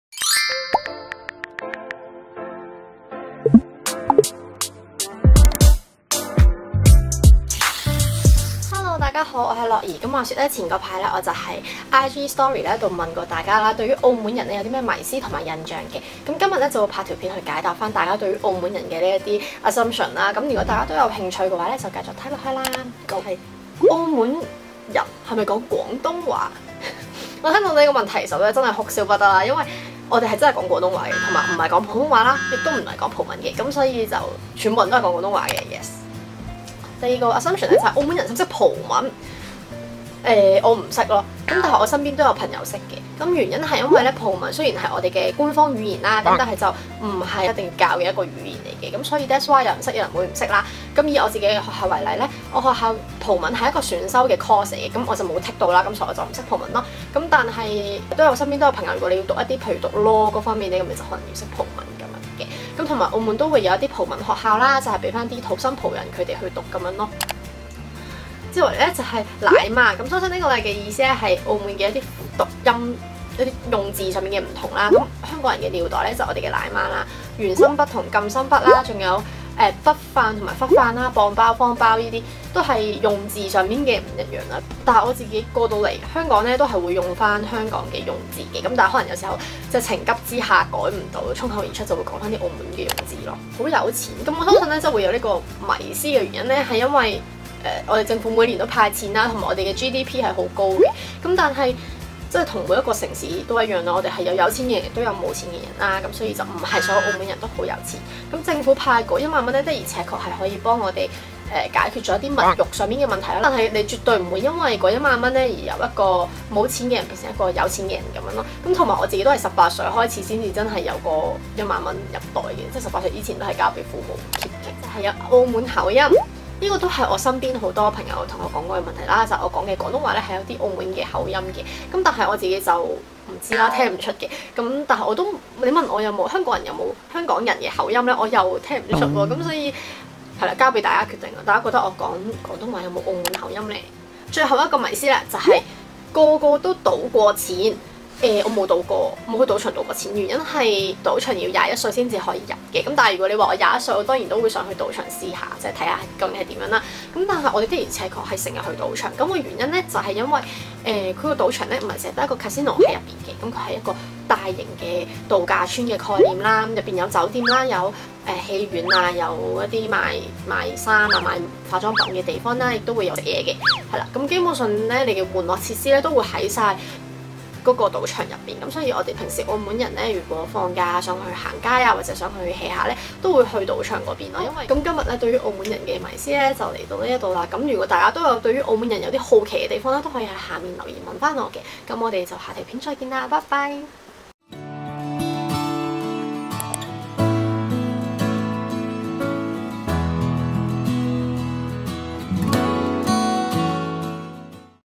超多有錢人？解答香港人對澳門人的六個迷思！🇲🇴
以上內容僅表達個人意見！！！
大家有咩好奇都可以comment問我😙
About me:
Camera: Canon M50 / IPhone13